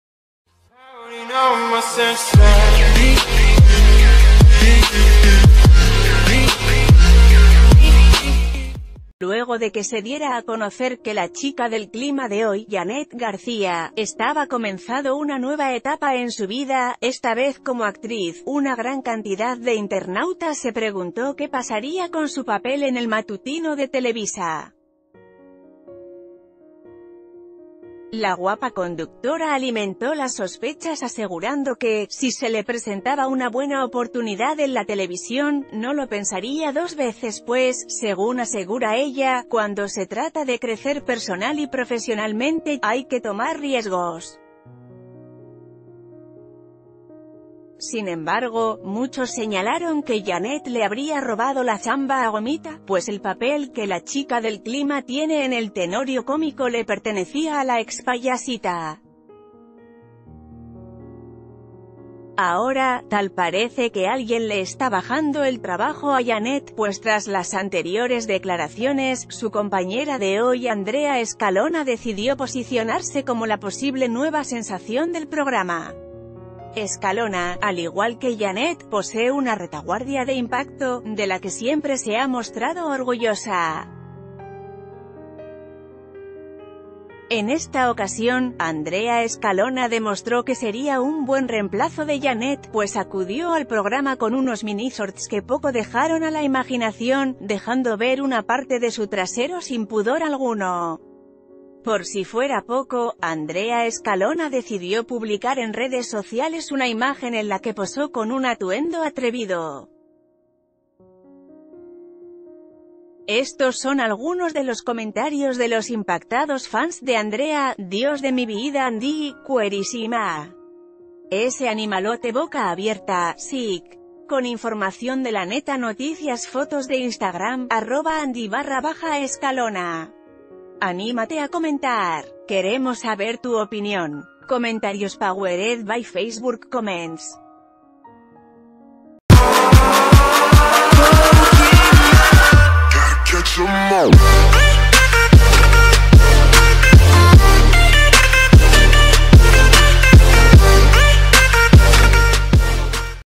Andrea Escalona reta a Yanet García con los cachetes de fuera en Hoy (FOTO)
Luego de que se diera a conocer que la chica del clima de Hoy, Yanet García, estaba comenzando una nueva etapa en su vida, esta vez como actriz, una gran cantidad de internautas se preguntó qué pasaría con su papel en el matutino de Televisa. La guapa conductora alimentó las sospechas asegurando que...